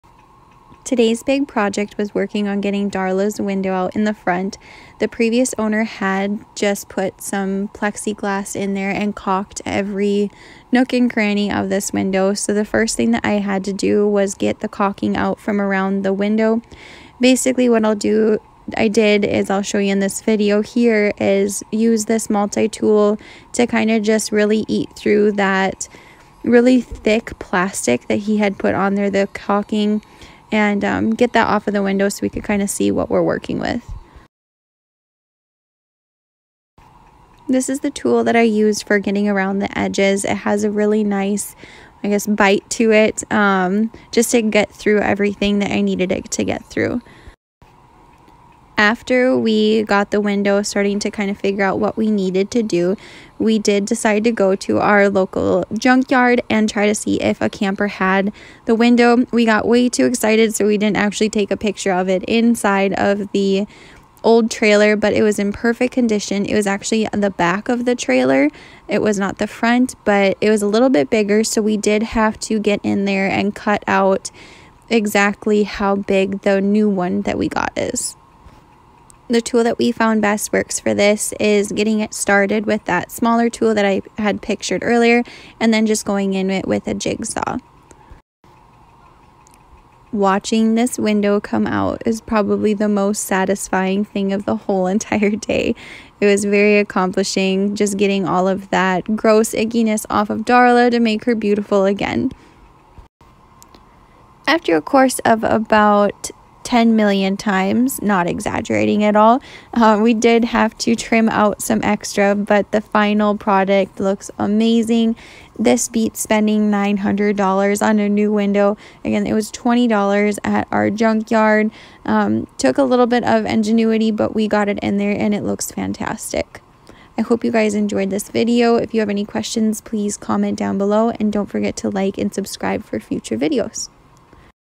New rv window for $20!!!
Are you lookin for cheaper options on how to replace your rv window or with what!? I replaced mine for $20!!

I didn’t showin the video because we are doing it after we paint this but caulk around the window before you secure it on the inside of the window frame and then on the outside of the frame.